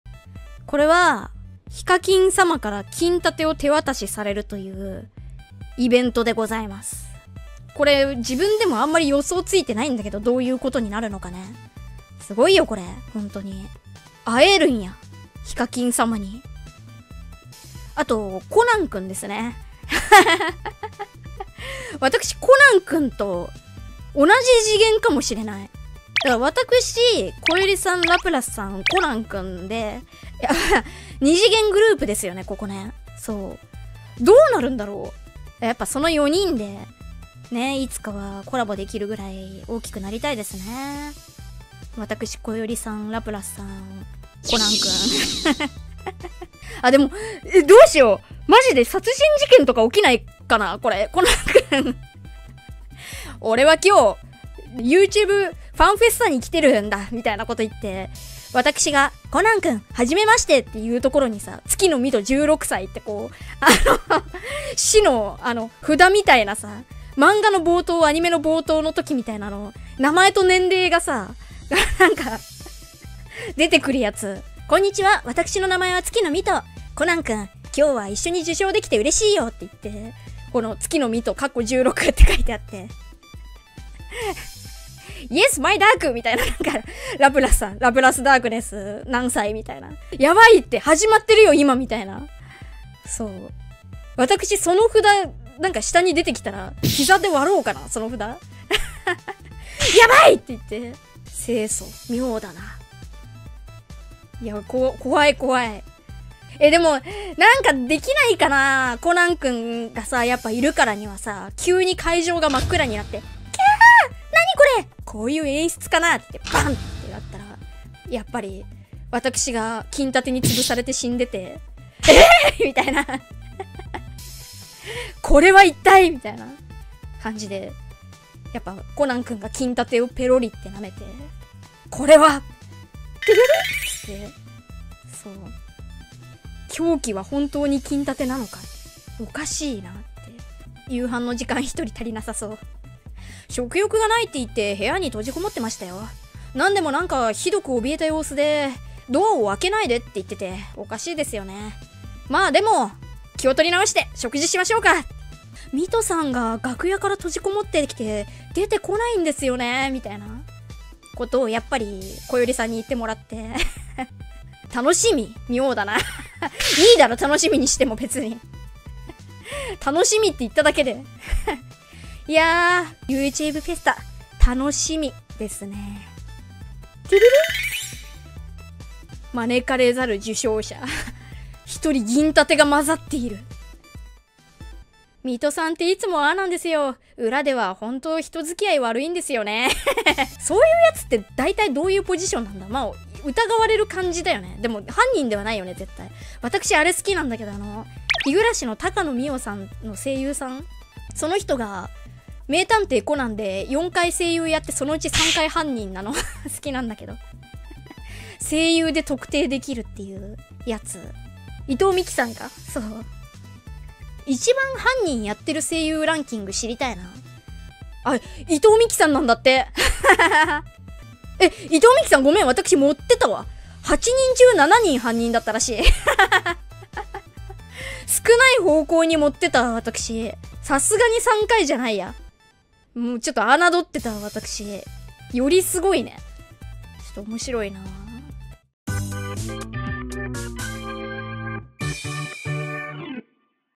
在YT fest名單中驚見米花死神的美兔，開始了O人事件的幻想【月ノ美兎】【彩虹社中文】【vtuber中文】
音樂、音效素材：
OtoLogic

圖片素材：
マンガパーツSTOCK